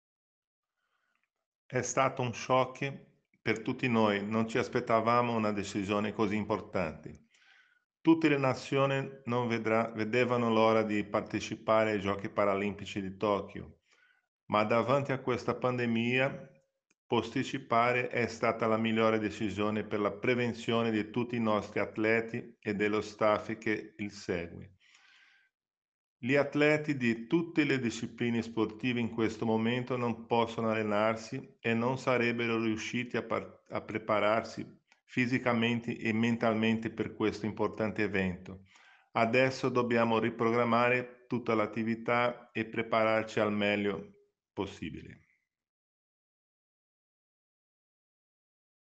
Tokyo 2020: le parole di Amauri Ribeiro sul rinvio delle Olimpiadi di Tokyo 2020
Ascoltiamo le parole del Tecnico della Nazionale femminile di sitting volley Amauri Ribeiro sul rinvio dei Giochi Olimpici di Tokyo 2020